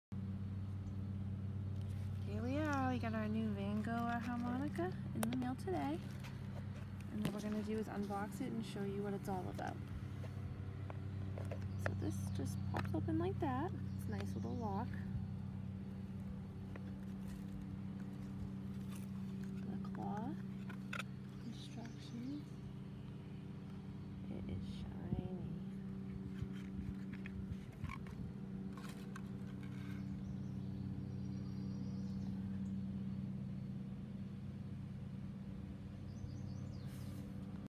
harmonica unboxing Vangoa Music
Vangoa harmonica unboxing - gorgeous gold color and hard case to store in. Great price from vangoa for another quality instrument. Get yours at Vangoa - Blues Deluxe 10 Hole Diatonic Harmonica with Case - Key of C (Golden)